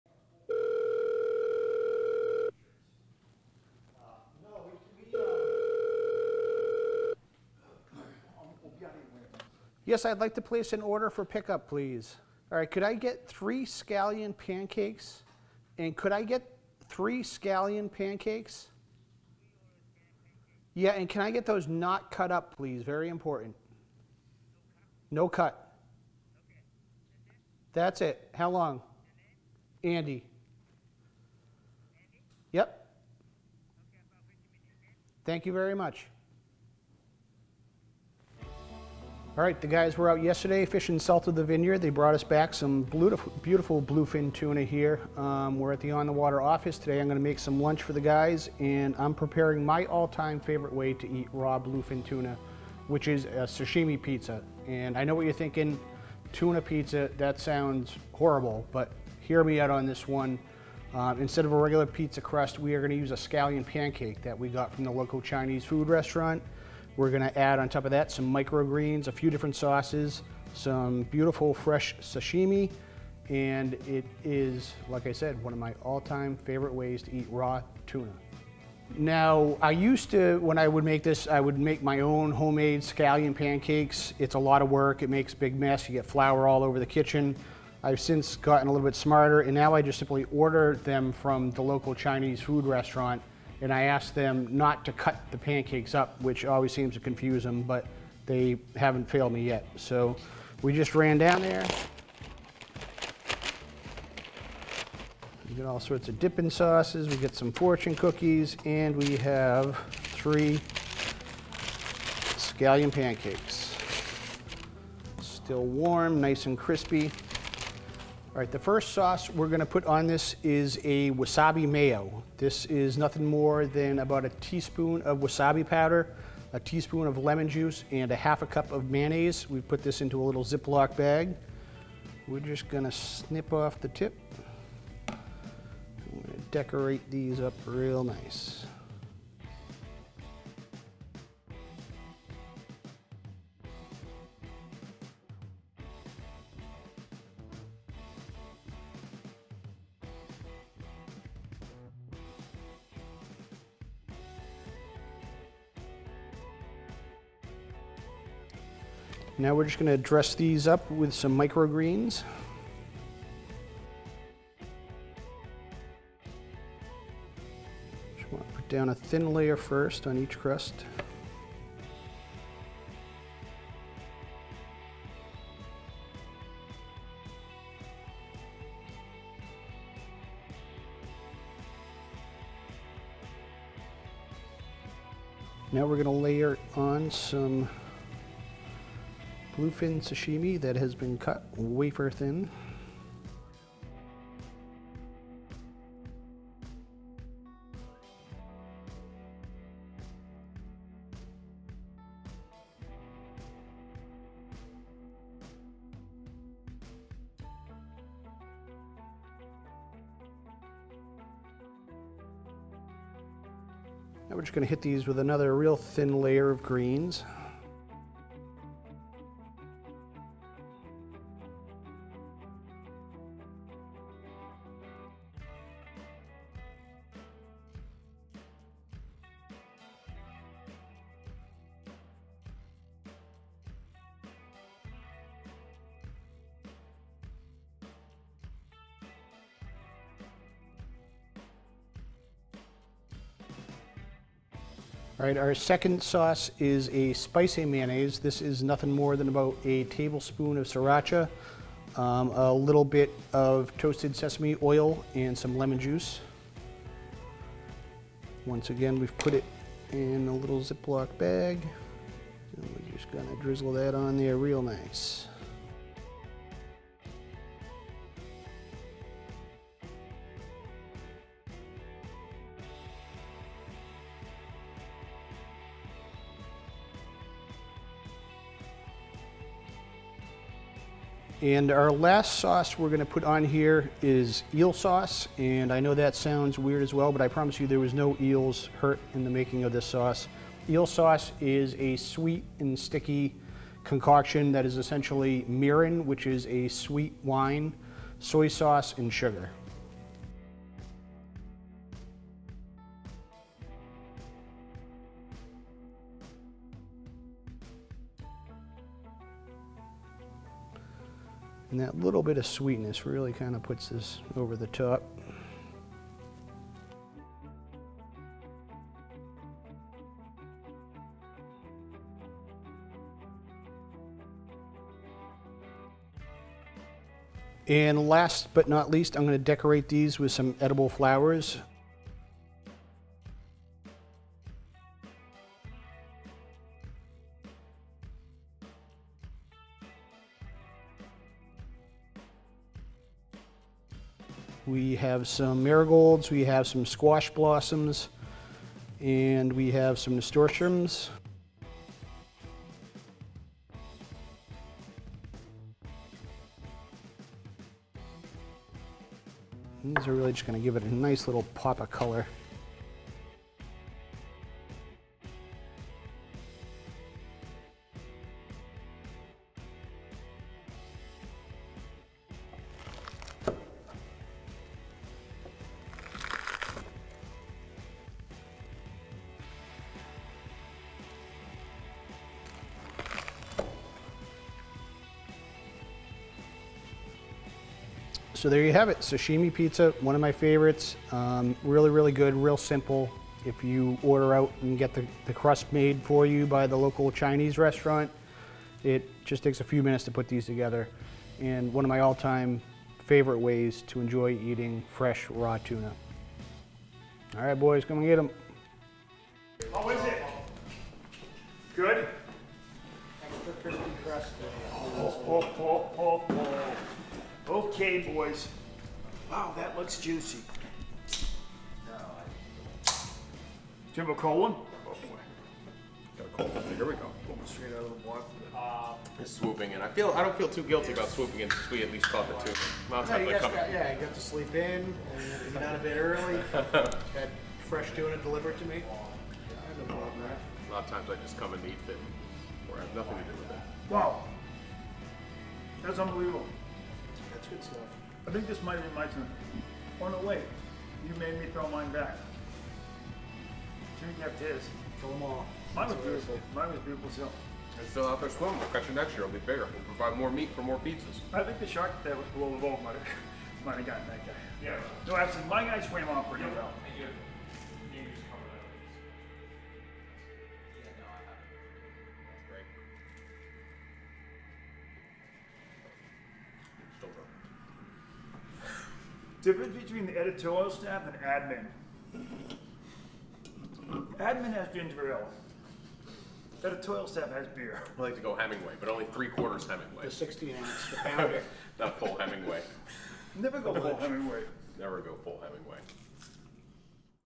How To Make Sashimi Pizza | Bluefin Tuna Recipe
When the tuna gods smile upon the OTW crew, our favorite way to prepare the bounty of bluefin, yellowfin, or bigeye is to make "Sashimi Pizza."

Instead of tomato sauce and mozzarella, this pizza begins with a scallion pancake "crust," sriracha, eel sauce, and wasabi, microgreens, and, of course, thinly sliced raw tuna.

Andy Nabreski describes how to quickly put together this flavorful dish that is certain to be a new favorite among you and your fishing buddies.